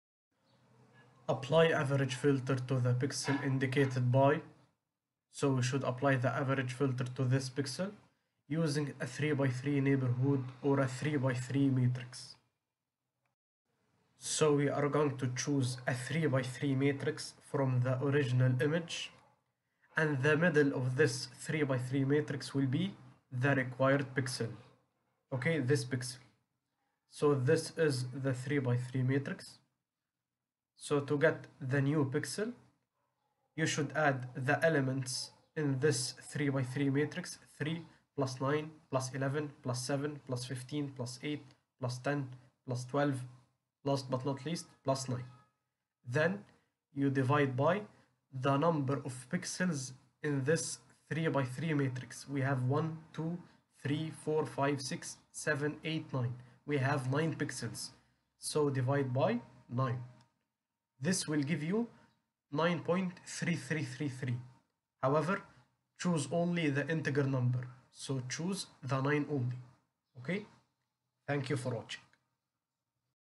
AVERAGE FILTER IN DIGITAL IMAGE PROCESSING SOLVED EXAMPLE
How to apply Average Filter method In Digital Image Processing